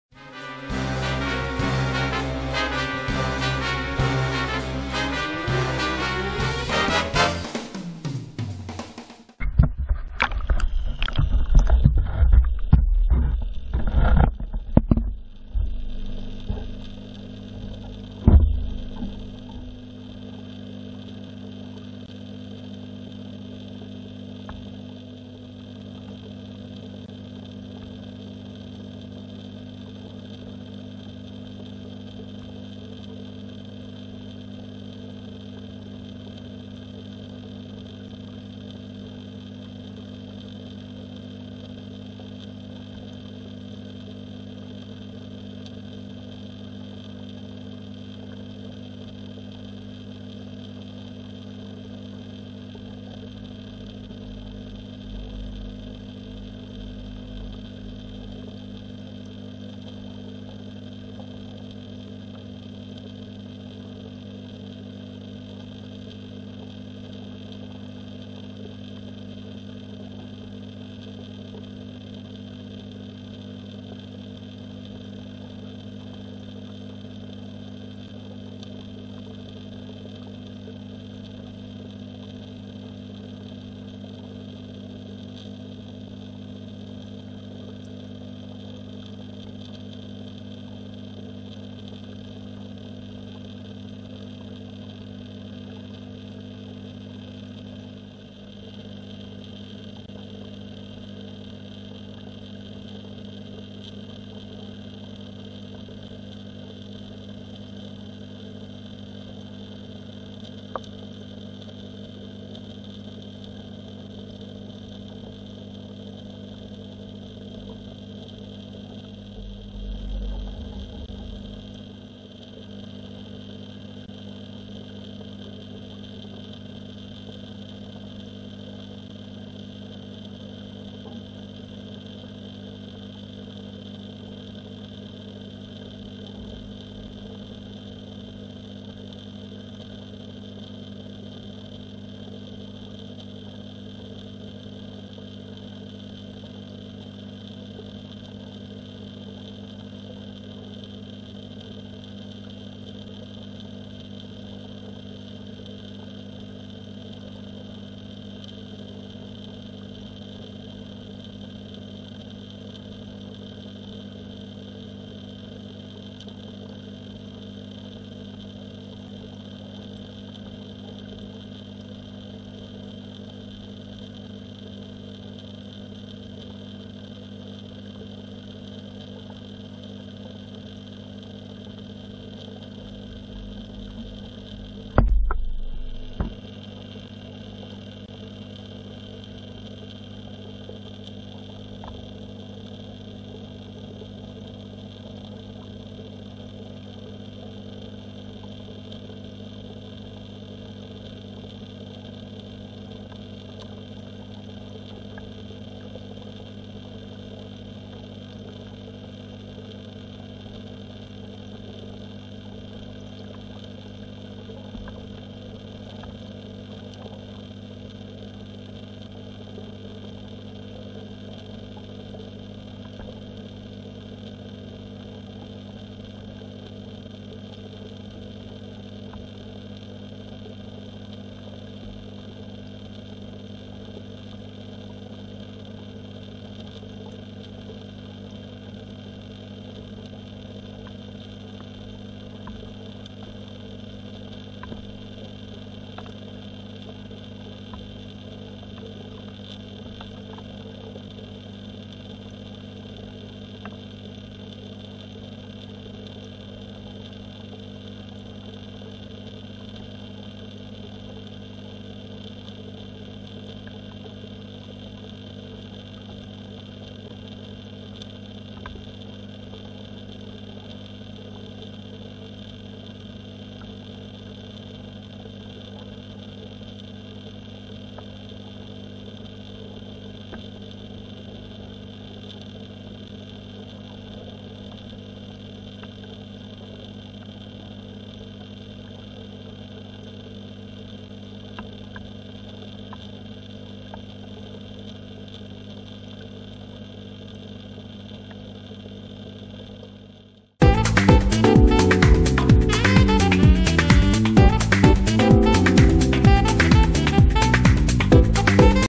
My Gold Fish
Testing my GoPro Hero3 in the back yard pond